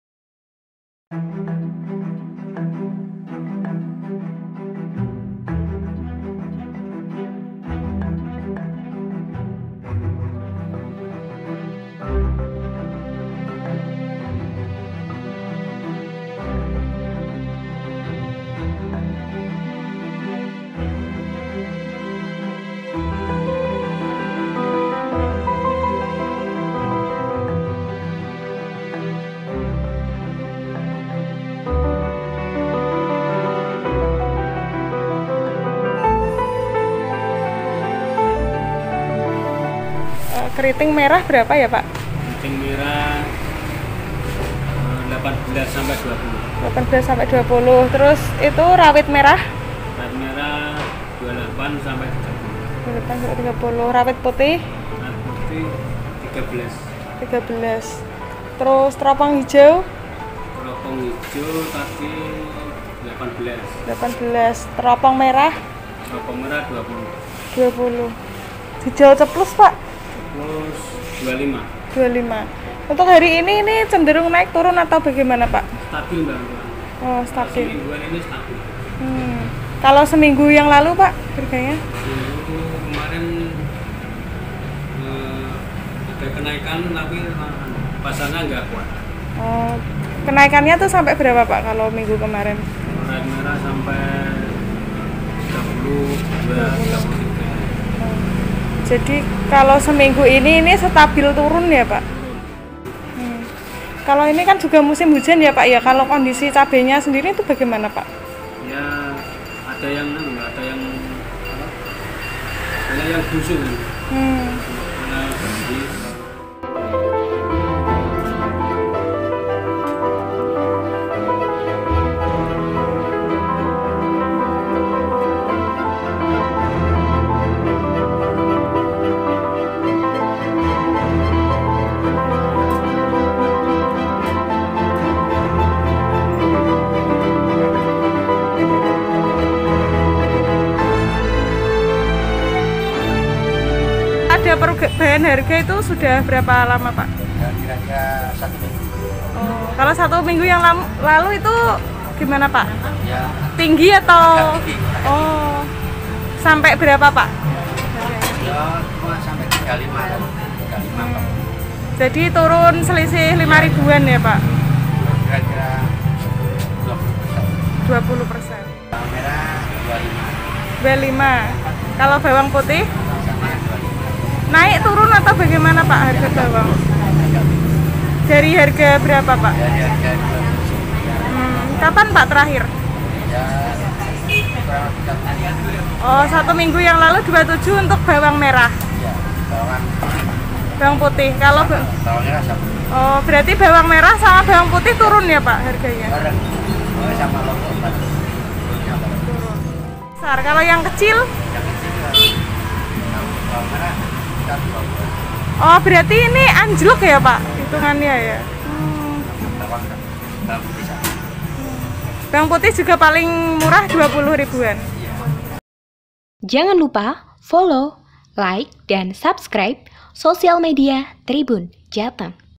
Update Harga Cabai di Kota Semarang Rp 28 Ribu Per Kg
Harga cabai di Kota Semarang terpantau stabil dalam sepekan terakhir.

Stabilnya harga cabai saat ini berada di angka rendah, setelah sempat naik lebih dari satu minggu lalu dengan harga tertinggi mencapai Rp 35 ribu per kilogram.

Harga tersebut kemudian turun dan stabil, dengan harga tertinggi rawit merah di kisaran Rp 28 ribu-Rp 30 ribu per kilogram.